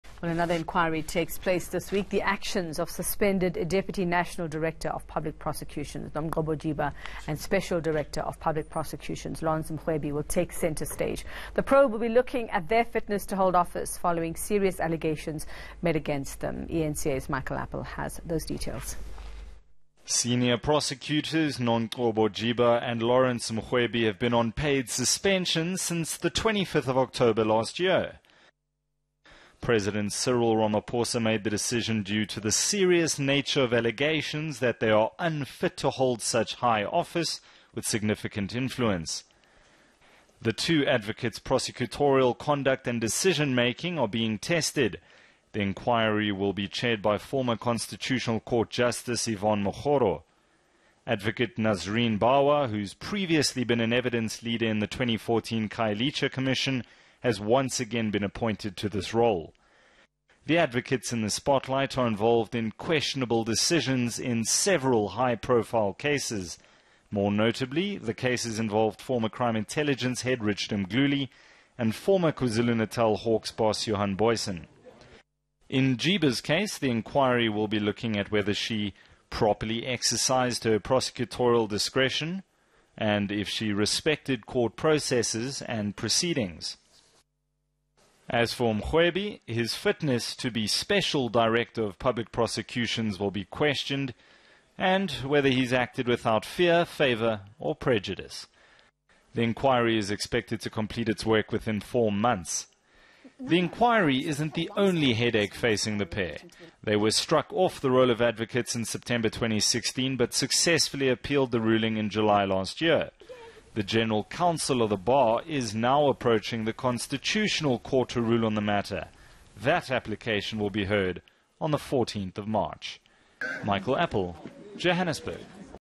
Jiba Mrewbi inquiry to start
The actions of suspended Deputy National Director of Public Prosecutions Nomgcobo Jiba and Special Director of Public Prosecutions Lawrence Mrwebi will take centre stage. eNCAOnline MorningNewsToday Courtesy